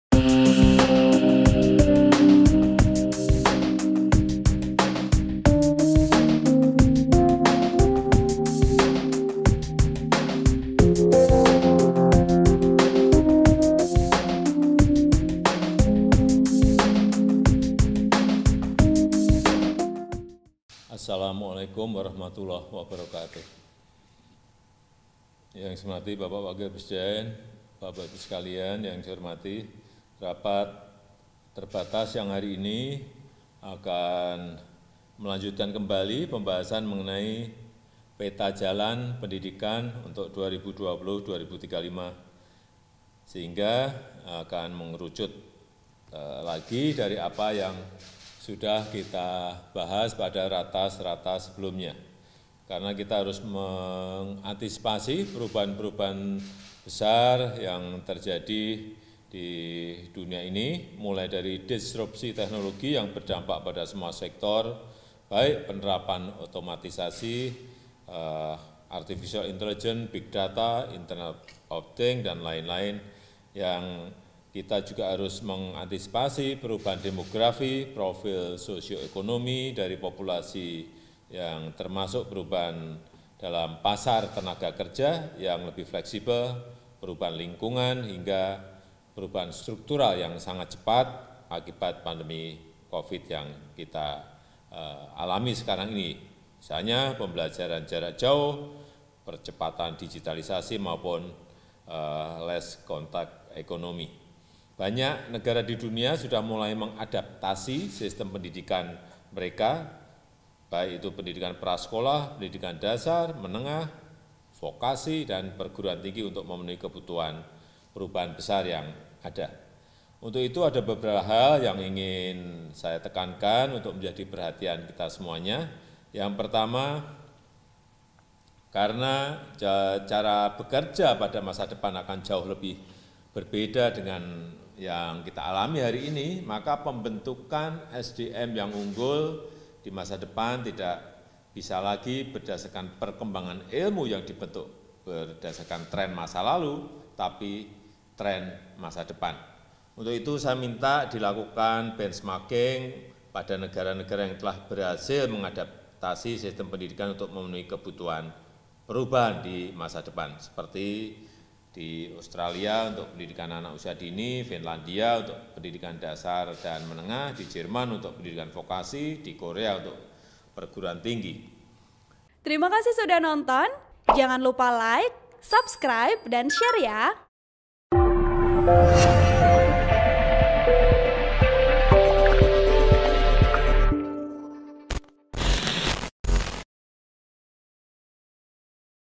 Banyak Perubahan Terjadi, Jokowi: Pendidikan Harus Adaptif
TRIBUN-VIDEO.COM - Presiden Joko Widodo meminta jajarannya untuk menyiapkan sistem pendidikan yang adaptif terhadap perubahan.

Hal itu disampaikan Presiden Jokowi saat membuka rapat terbatas tentang peta jalan pendidikan Indonesia melalu konferensi video, Kamis (4/6/2020).

Ia meminta sistem pendidikan adaptif terhadap perubahan yang diakibatkan disrupsi teknologi dan pandemi Covid-19 yang mengharuskan pembelajaran dilakukan secara jarak jauh.

"Kita harus antisipasi perubahan-perubahan besar yang terjadi di dunia ini mulai dari disrupsi teknologi yang berdampak pada semua sektor," ujar Presiden Jokowi.

"Hingga perubahan struktural yang sangat cepat akibat pandemi Covid-19 yang kita alami sekarang ini. Misalnya pembelajaran jarak jauh, percepatan digitalisasi, maupun less contact economy," lanjut dia.
Untuk itu, ia meminta jajarannya mempelajari upaya negara lain yang sukses mereformasi sistem pendidikannya di kala disrupsi teknologi sekaligus pandemi Covid-19.

Beberapa di antaranya Australia untuk pendidikan anak usia dini, Finlandia untuk pendidikan dasar dan menengah, Jerman untuk pendidikan vokasi, serta Korea Selatan untuk pendidikan tinggi.
"Banyak negara di dunia sudah mulai mengadaptasi sistem pendidikan mereka, baik itu pendidikan prasekolah, pendidikan dasar, menengah, vokasi dan perguruan tinggi untuk memenuhi kebutuhan perubahan besar yang ada," lanjut Presiden Jokowi.

Artikel ini telah tayang di Kompas.com dengan judul "Jokowi Minta Sistem Pendidikan Adaptif dengan Perubahan",
Penulis : Rakhmat Nur Hakim
Editor : Fabian Januarius Kuwado